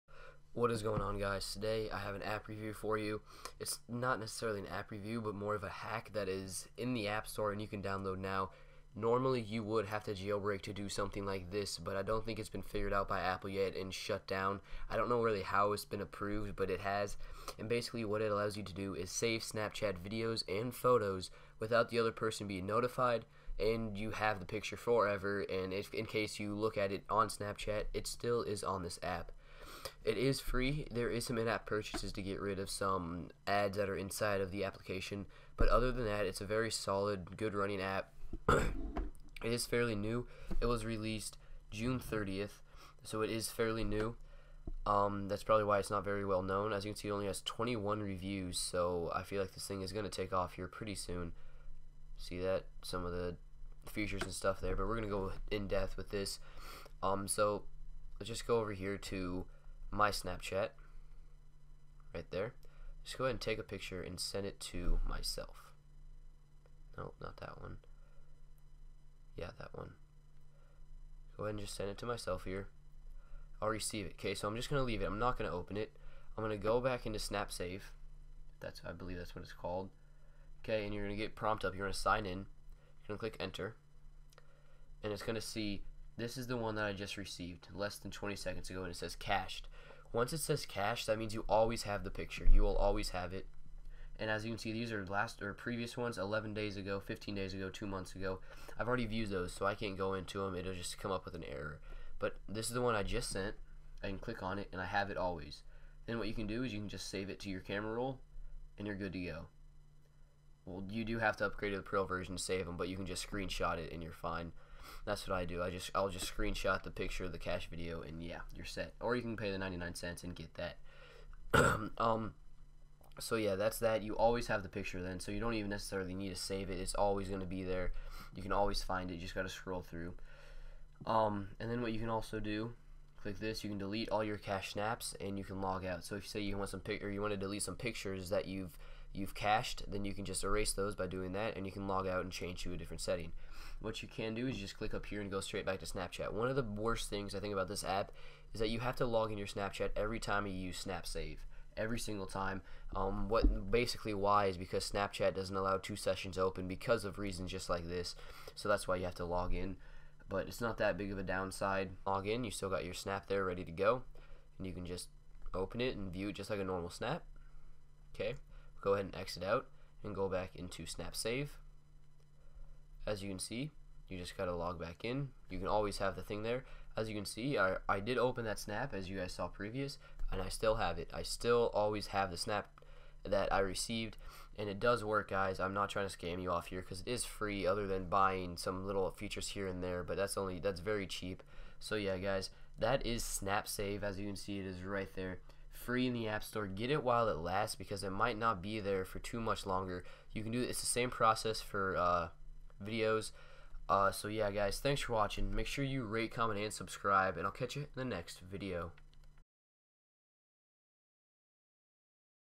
Save SnapChat Photos and Videos With App Called Snap Save. NO JAILBREAK
This app allows you to access videos and photos from snapchat forever once you cache them. You can save them directly to your photos app or just keep them in Snap Save.